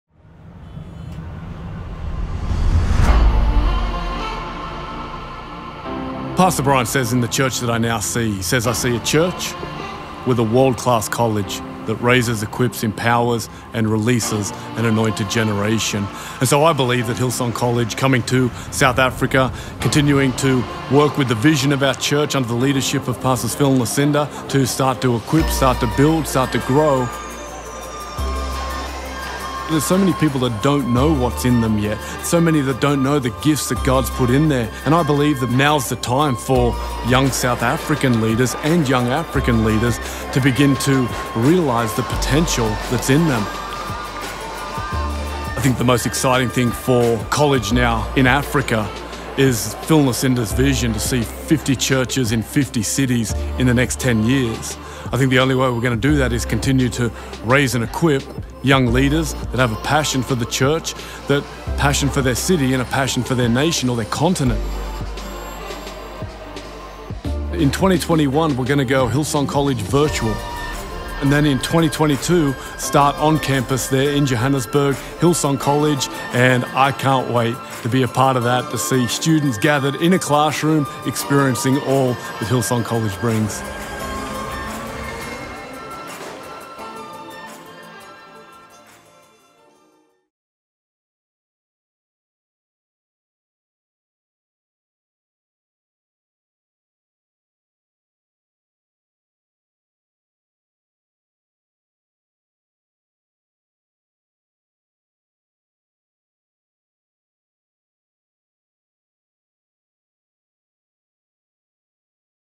Hillsong College Africa — The Vision Continues | Special Announcement
Announcing Hillsong College Africa.

We’re excited to announce that Hillsong College courses will be available on campus in South Africa! In all that we do, our mission is to raise, equip, empower and release anointed leaders to lead lives of impact. Express your interest below to learn more and to take your next step in pursuing the call of God on your life.

The Vision Continues — Exciting News. New Plans. Fresh Vision.